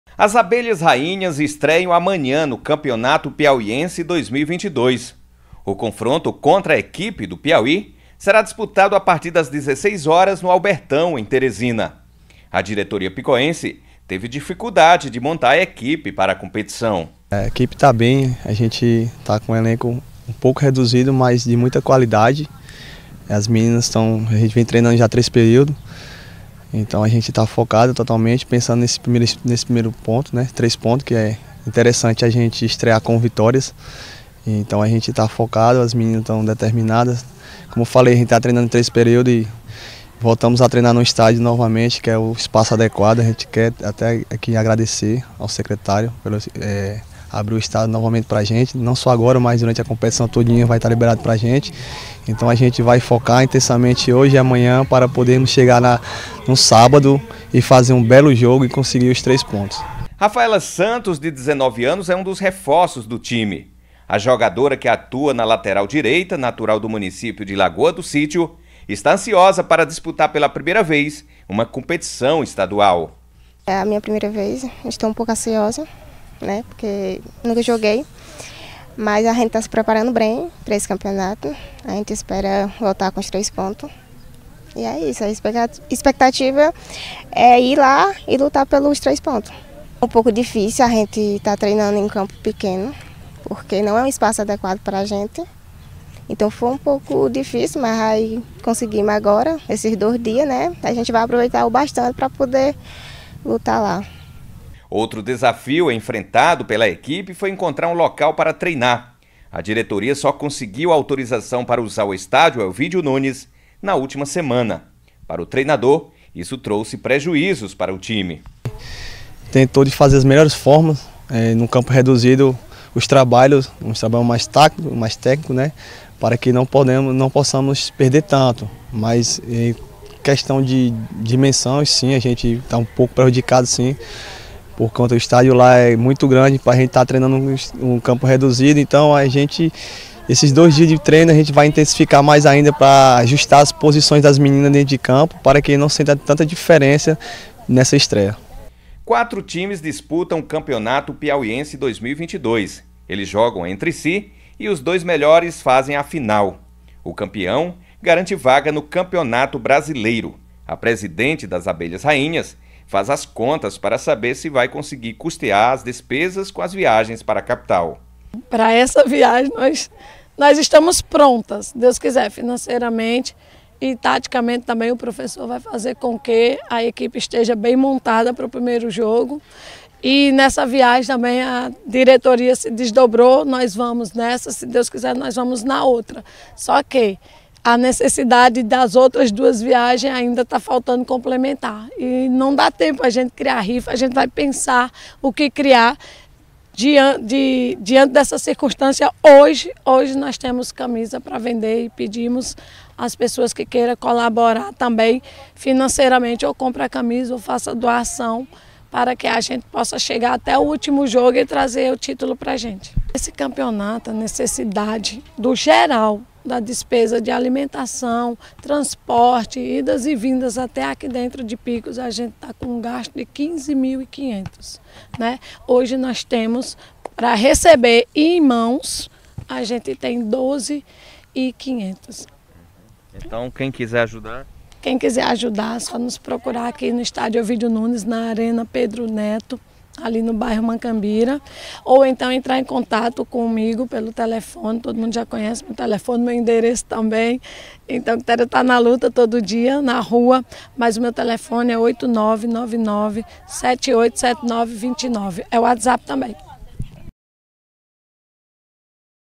AS ABELHAS RAINHAS ESTREIAM NO CAMPEONATO PIAUIENSE
VT produzido e veiculado pela TV Picos - Canal 13. Reportagem de Jonas Rocha.